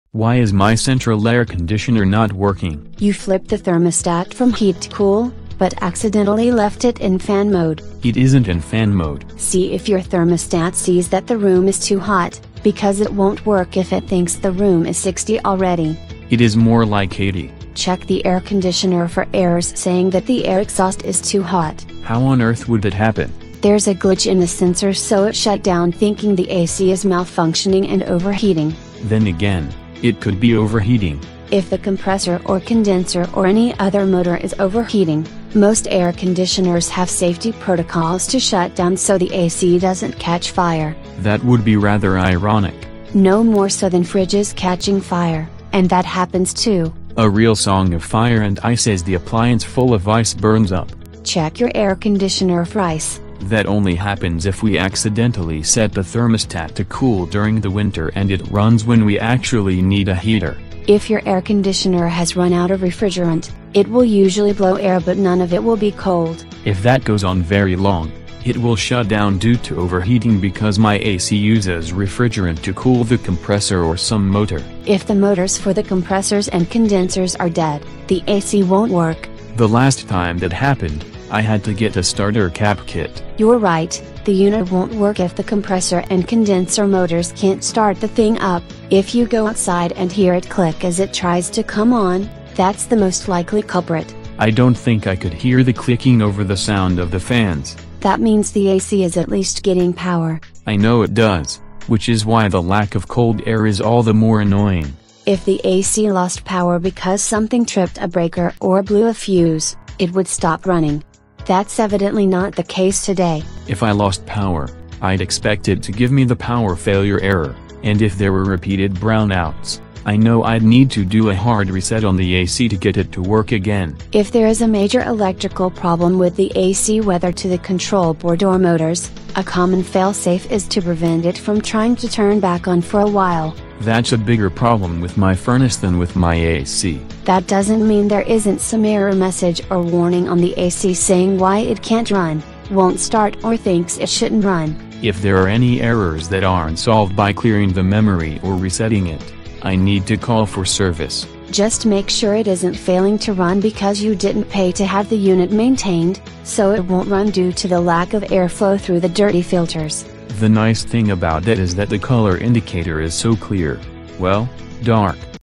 Why is My Central Air Conditioner Not Working?
You flipped the thermostat from heat to cool, but accidentally left it in fan mode.

It isn't in fan mode.

See if your thermostat sees that the room is too hot, because it won't work if it thinks the room is 60 already.

It is more like 80.

Check the air conditioner for errors saying that the air exhaust is too hot.

How on Earth would that happen?

There's a glitch in the sensor so it shut down thinking the AC is malfunctioning and overheating.

Then again, it could be overheating.

If the compressor or condenser or any other motor is overheating, most air conditioners have safety protocols to shut down so the AC doesn't catch fire.

That would be rather ironic.

No more so than fridges catching fire, and that happens too.

A real song of fire and ice as the appliance full of ice burns up.

Check your air conditioner for ice.

That only happens if we accidentally set the thermostat to cool during the winter and it runs when we actually need a heater.

If your air conditioner has run out of refrigerant, it will usually blow air but none of it will be cold.

If that goes on very long, it will shut down due to overheating because my AC uses refrigerant to cool the compressor or some motor.

If the motors for the compressors and condensers are dead, the AC won't work.

The last time that happened, I had to get a starter cap kit.

You're right, the unit won't work if the compressor and condenser motors can't start the thing up. If you go outside and hear it click as it tries to come on, that's the most likely culprit.

I don't think I could hear the clicking over the sound of the fans.

That means the AC is at least getting power.

I know it does, which is why the lack of cold air is all the more annoying.

If the AC lost power because something tripped a breaker or blew a fuse, it would stop running. That's evidently not the case today.

If I lost power, I'd expect it to give me the power failure error, and if there were repeated brown outs, I know I'd need to do a hard reset on the AC to get it to work again.

If there is a major electrical problem with the AC whether to the control board or motors, a common fail safe is to prevent it from trying to turn back on for a while.

That's a bigger problem with my furnace than with my AC.

That doesn't mean there isn't some error message or warning on the AC saying why it can't run, won't start or thinks it shouldn't run.

If there are any errors that aren't solved by clearing the memory or resetting it, I need to call for service.

Just make sure it isn't failing to run because you didn't pay to have the unit maintained, so it won't run due to the lack of air flow through the dirty filters.

The nice thing about that is that the color indicator is so clear, well, dark.